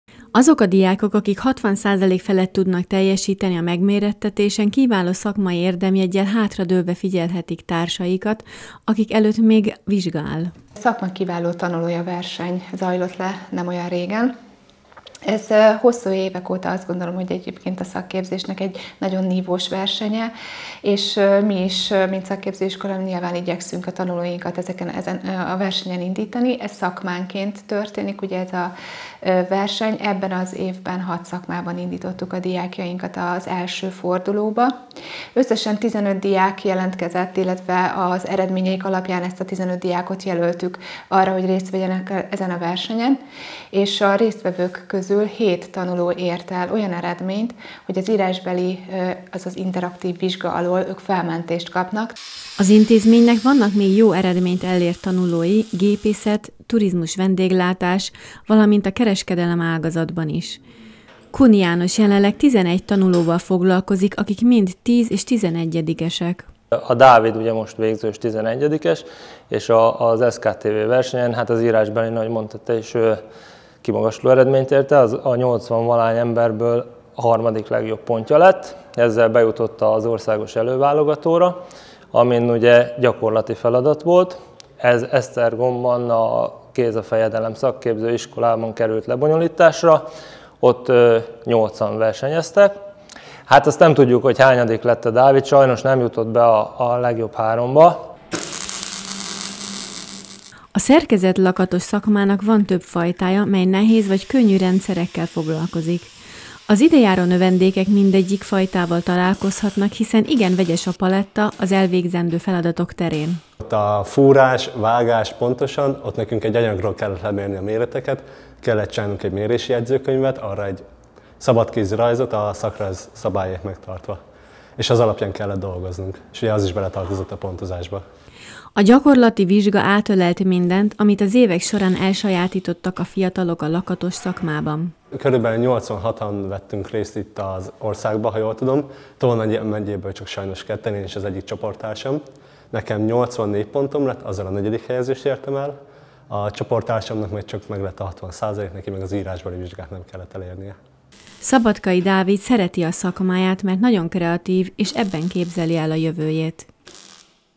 Szakma Kiváló Tanulója - kitűnő eredmények a Tolna vármegyei diákoktól
A dunaföldvári Magyar László Szakképző Iskola két diákja is kiemelkedő eredményeket ért el a Szakma Kiváló Tanulója versenyen.